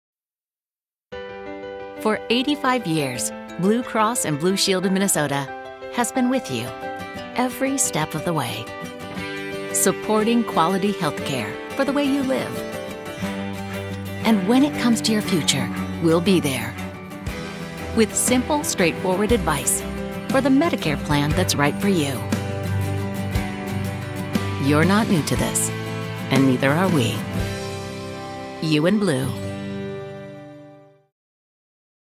Together / BlueCross BlueShield / Evergreen Pictures
A simple but elegant treatment to promote healthcare. Another fun collaboration with Wunderman and Evergreen Pictures. A great example of a spot that uses almost all of our specialties to deliver a multi-spot campaign that is truly special.